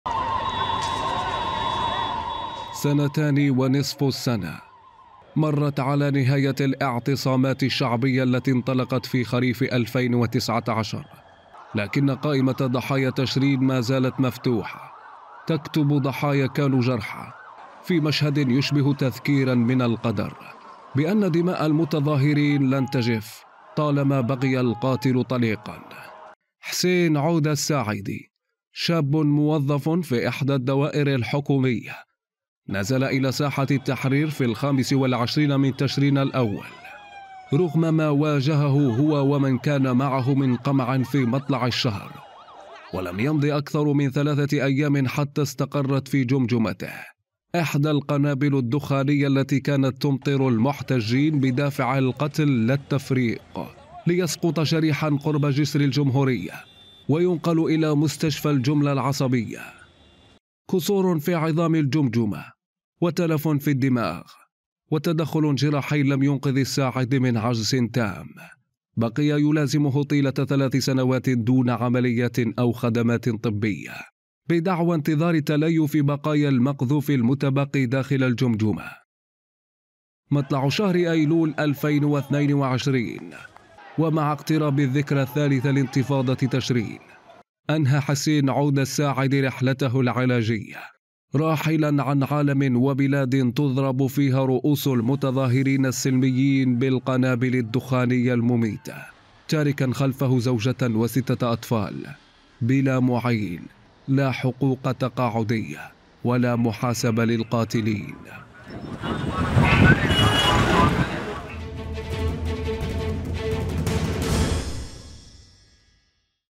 انتفاضة تشرين في العراق تسجل ضحية جديدة بعد 3 سنوات | تقرير
مع اقتراب الذكرى الثالثة لانتفاضة تشرين .. تسجل الأخيرة اسماً جديداً في قائمة الضحايا بعد نحو ثلاث سنوات من اصابته بقنبلة دخانية ليكون اخر ضحايا العنف الذي رافق احداث خريف 2019.
تقرير - غرفة الأخبار
موقع قناة التغيير الفضائية الالكتروني